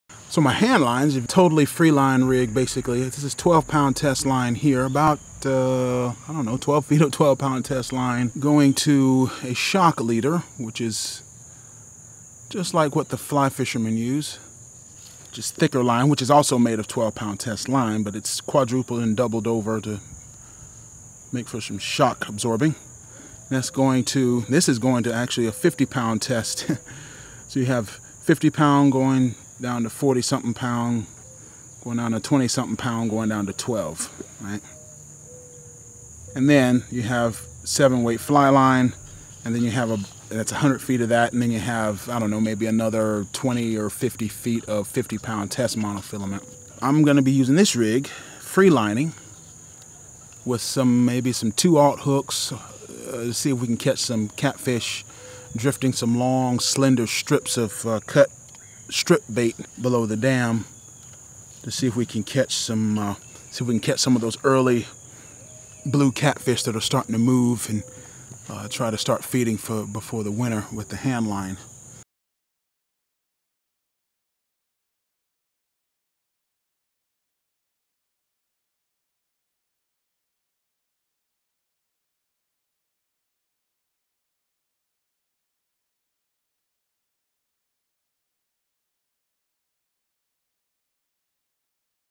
Freeline Fishing Rig for Handlines
Freeline fishing for handlines is fun. It's a simple handline fishing rig that doesn't require weight. Hand line rigs are often best in drift fishing. Hand line trolling rig can tire your hands faster.

FREE STUFF
Trifecta of Making Horsepower PDF